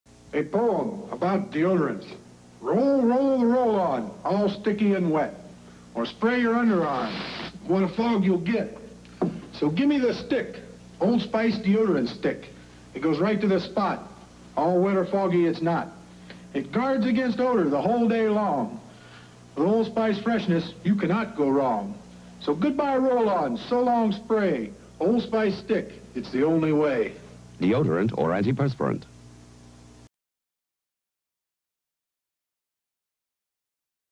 Old Spice Deodorant Commercial (Larry Csonka, 1978)
"A poem - about deodorant!"
Old Spice deodorant & antiperspirant commercial featuring football player Larry Csonka (Miami Dolphins). Spot aired December 1978. for 1970s pop culture fun.*